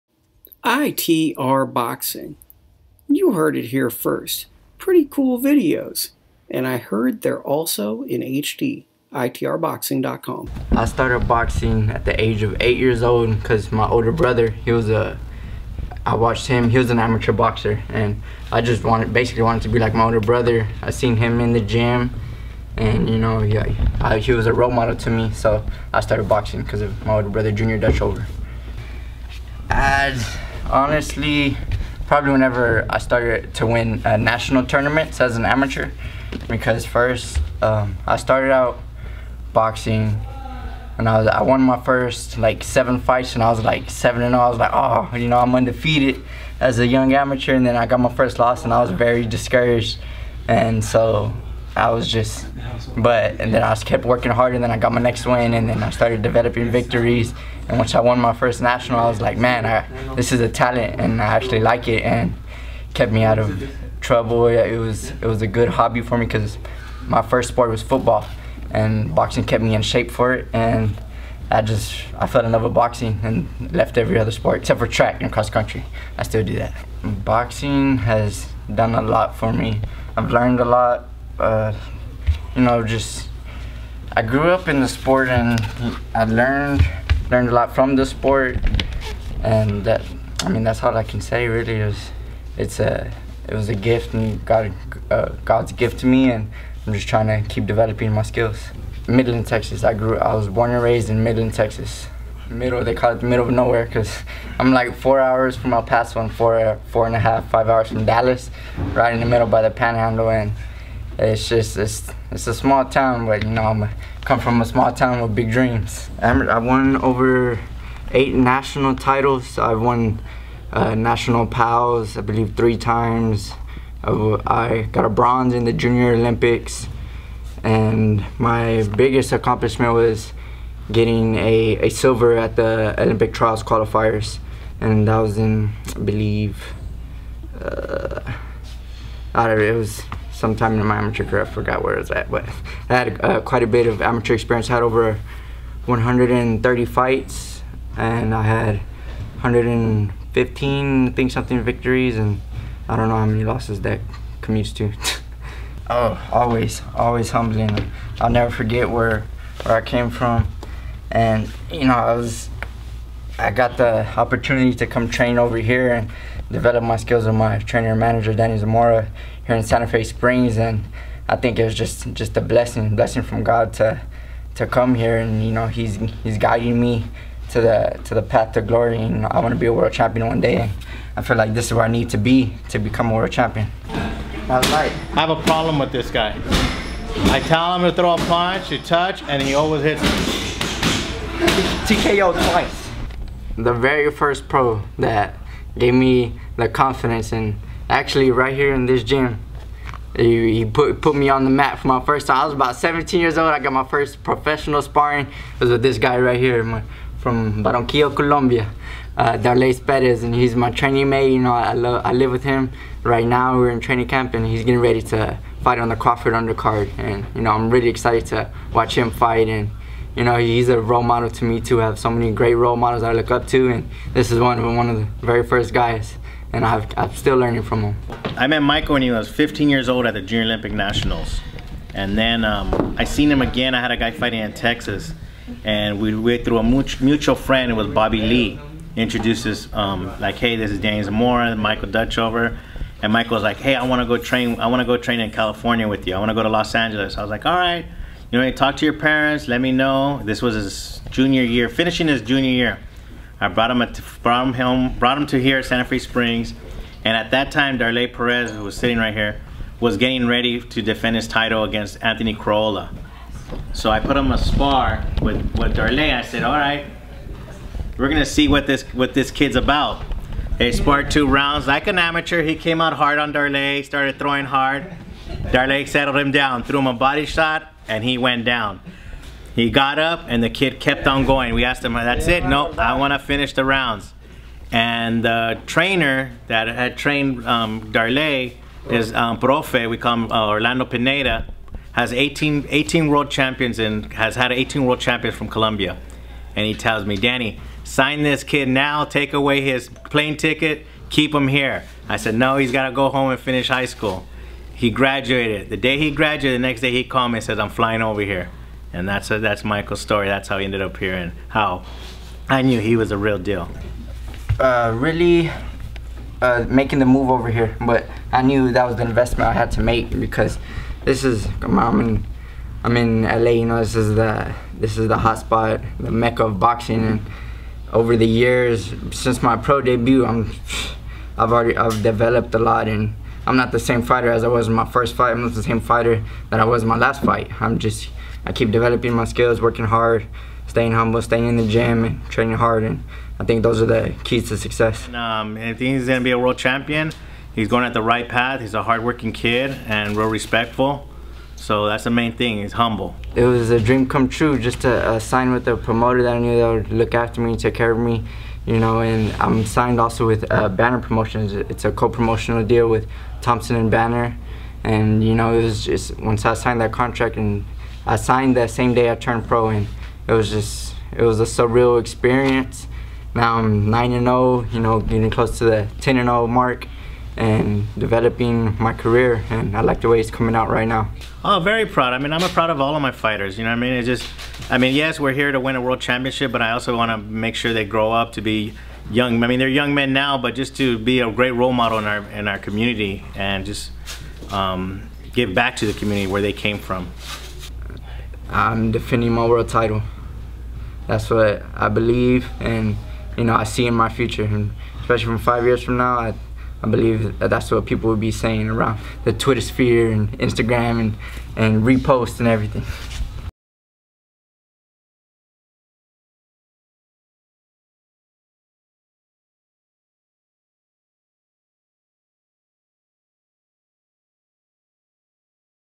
Michael Dutchover Documentary | Texas Boxer
Michael Dutchover is a highly decorated amateur boxer who turned professional with Thompson Boxing Promotions and Banner Boxing. Dutchover now trains in Sante Fe Springs, CA with his coach and manager Danny Zamora.

Occupation: Fighter is a hands-on look at the life and sacrifice that a fighter goes through for the average boxing fan to even form an opinion on them.